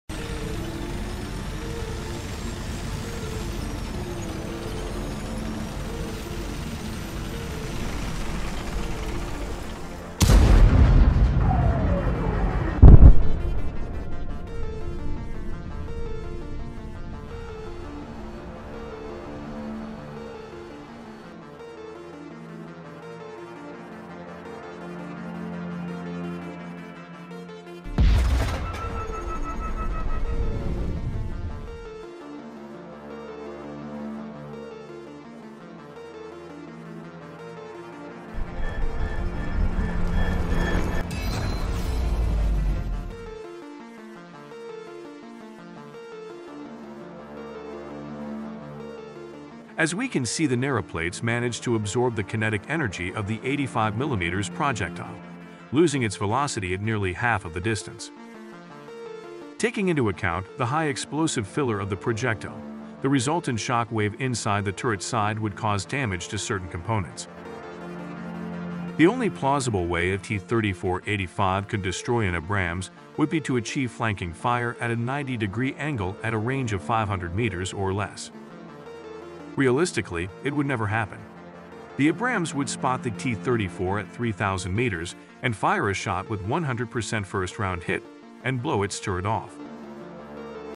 T-34-85 VS M1A1 Abrams | Armour Piercing Simulation
This simulation shows the impact of a 85mm BR-365K APHE-T being fired from a Soviet T-34-85 tank at a range of 0.23KM, targeting the turret side of a US M1A1HA Abrams tank.

As we can see the NERA plates managed to absorb the kinetic energy of the 85mm projectile, losing its velocity at nearly half of the distance. Taking into account the high explosive filler of the projectile, the resultant shock wave inside the turret side would cause damage to certain components.

The only plausible way a T-34-85 could destroy an Abrams would be to achieve flanking fire at a 90 degree angle at a range of 500 meters or less.

Realistically, it would never happen. The Abrams would spot the T-34 at 3000 meters and fire a shot with 100% first round hit and blow its turret off.